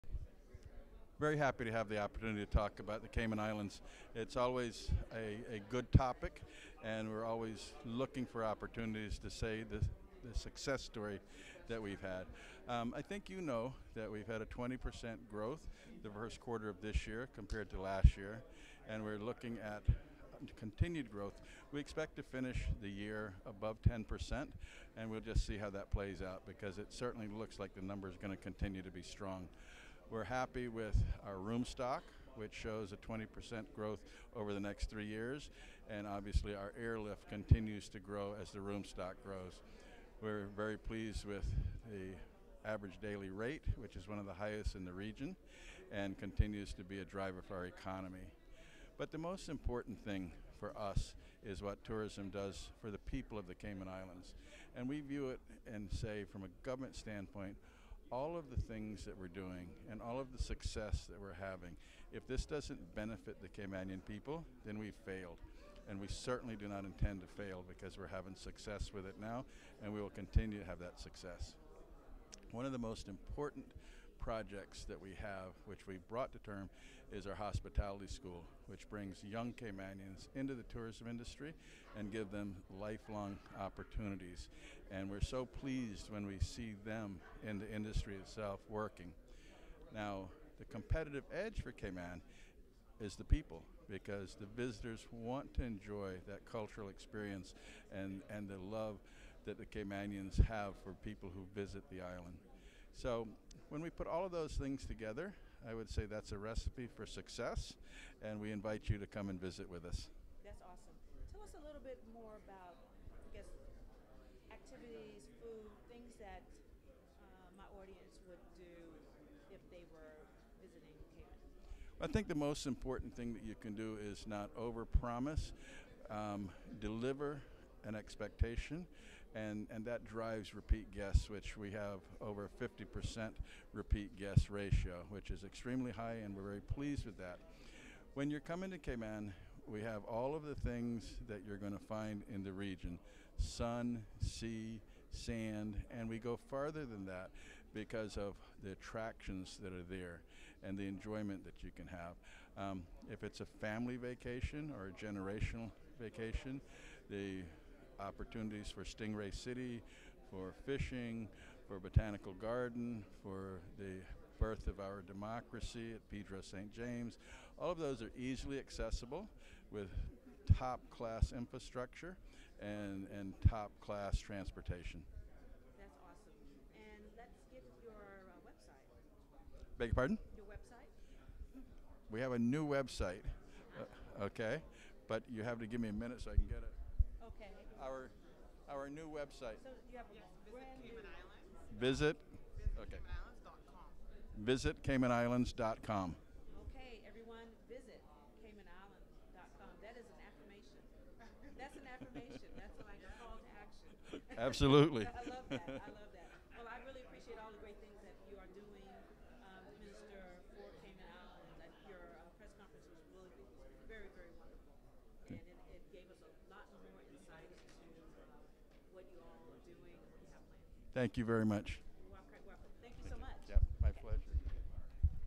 Caribbean Week NYC on Cultural Traveler UBCTV
The Cayman Islands did a great presentation during Caribbean Week. They are going through a major uptick in growth of the islands tourism. They have a lot to offer. Check out what the Honorable Moses Kirkconnell has to say about the Cayman Islands!